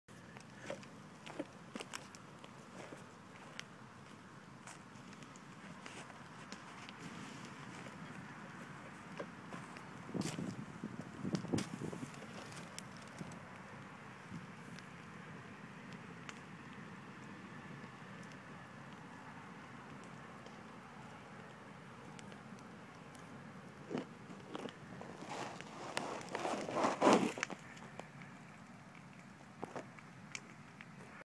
Pine knot shavings
Pine knot saw dust I threw into the campfire. Super hot super quick